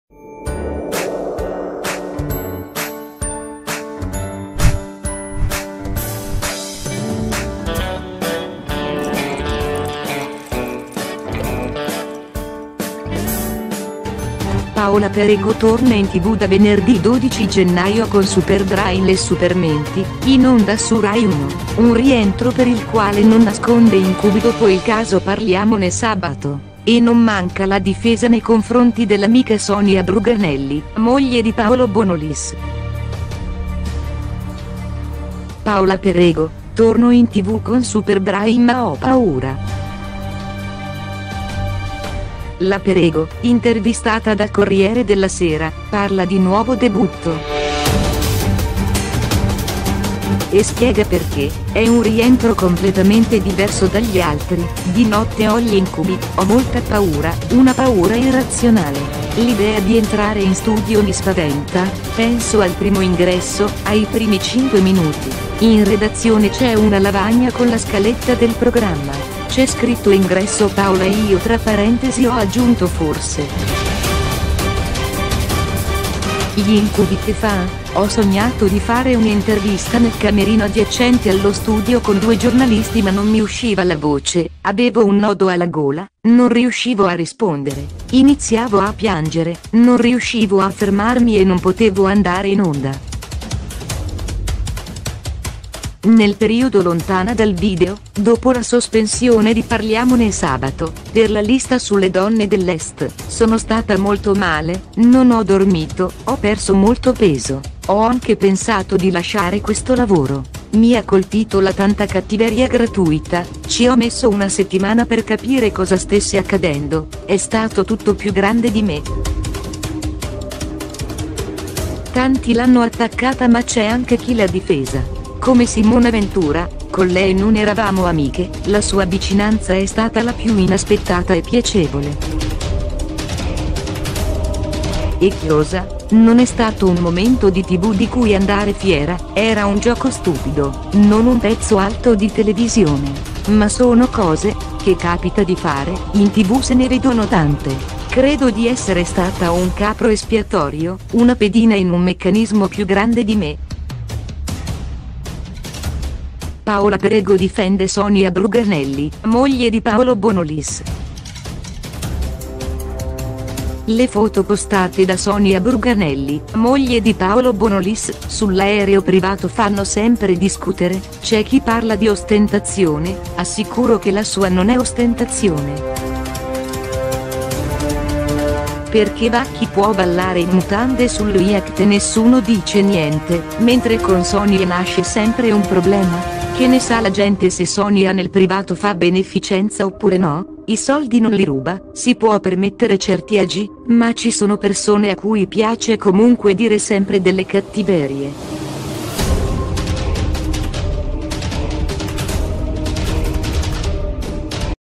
Paola Perego torna in tv: “Sono stata male, ho perso peso”. E ringrazia Simona Ventura
Paola Perego torna in tv da venerdì 12 gennaio con Superbrain-Le supermenti, in onda su Rai1. Un rientro per il quale non nasconde “incubi” dopo il caso-Parliamone sabato. E non manca la difesa nei confronti dell’amica Sonia Bruganelli, moglie di Paolo Bonolis.